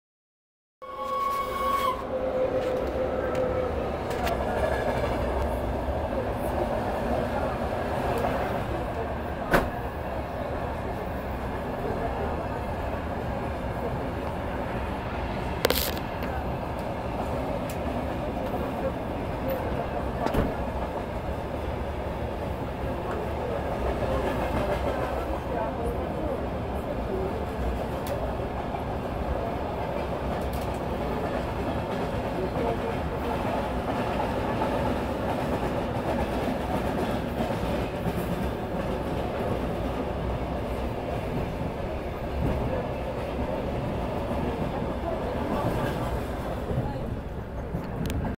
вл80к-151 з вантажним поїздом по ст. Ніжин CAM08049
17,06,2016 17ч

вл80к-151 с грузовым поездом по станции Нежин